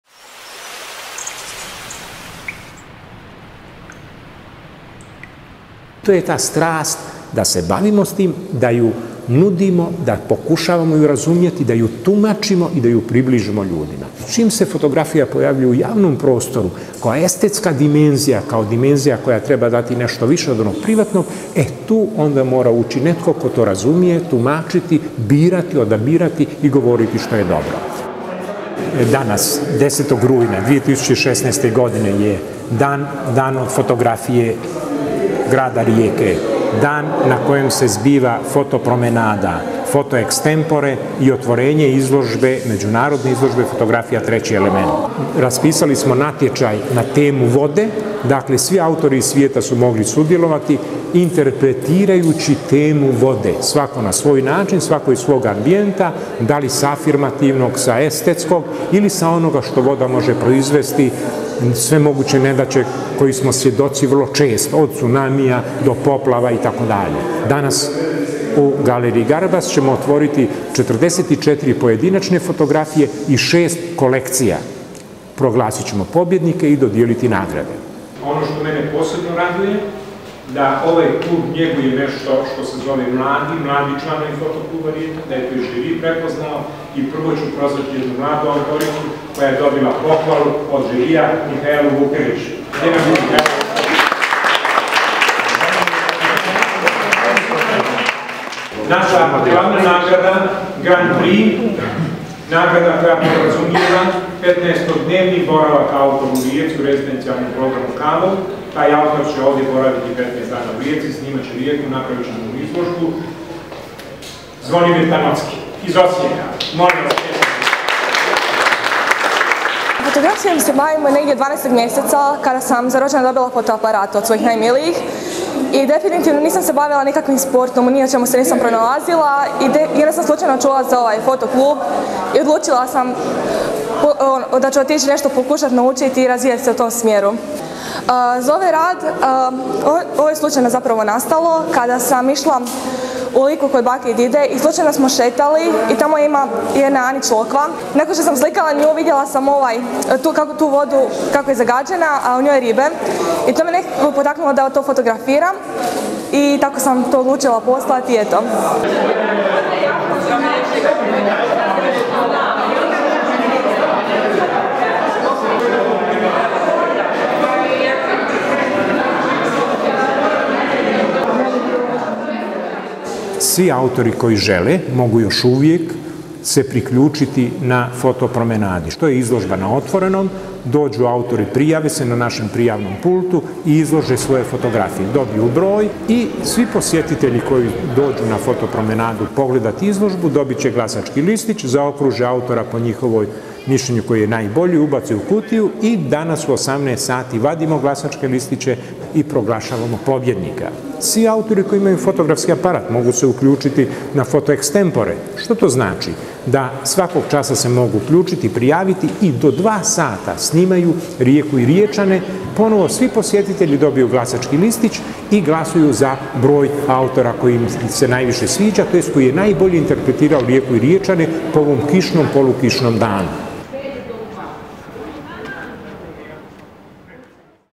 MojaRijeka.hr - PHOTORI 2016.
U subotu, 10. rujna, održan je sedmi zaredom riječki festival fotografije, PHOTORI 2016. u organizaciji Fotokluba Rijeka. Galerije Garbas ugostila je međunarodnu izložbu fotografija “Treći element”, a prostor Arheološkog parka postao je pozornica za izlagače na Fotopromenadi i Foto ex temporeu.